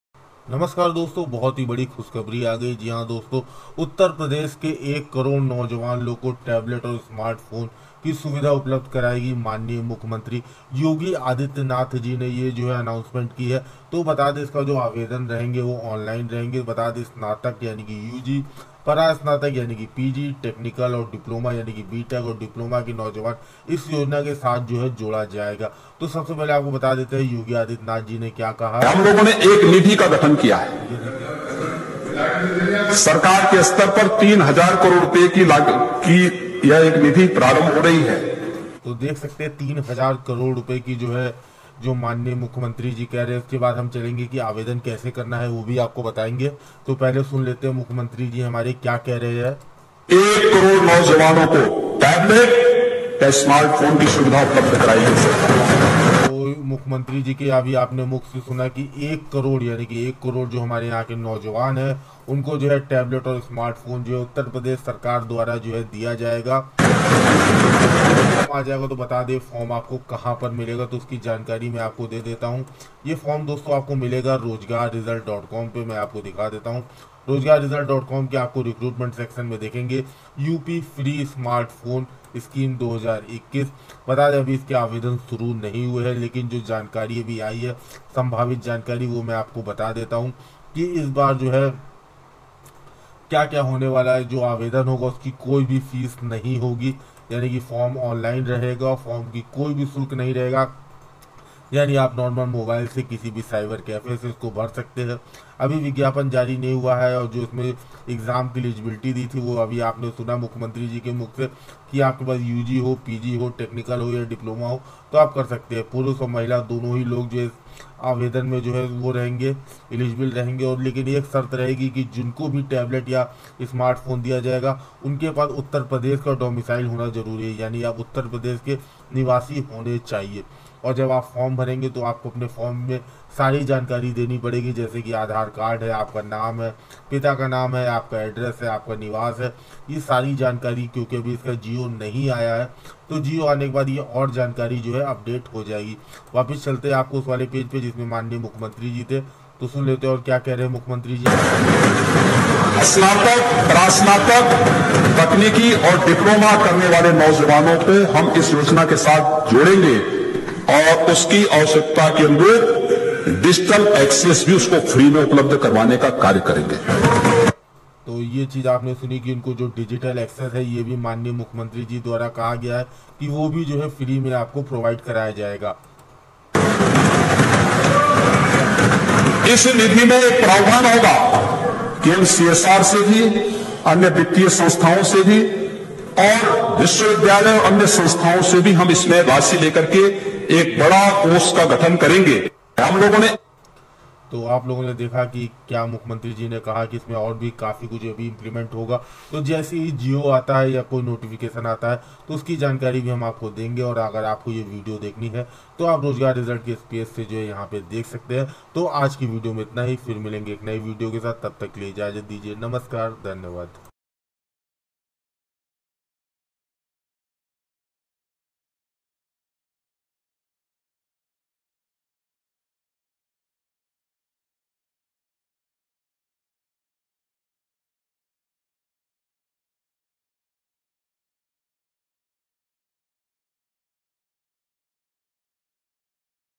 बहुत बड़ी खुशखबरी माननीय योगी जी द्वारा मुक्त लैपटॉप वितरण स्तानक परास्नातक तकनीकी और डिप्लोमा।
बहुत बड़ी खुशखबरी माननीय योगी जी द्वारा मुक्त लैपटॉप वितरण स्तानक परास्नातक तकनीकी और डिप्लोमा वालो के लिए।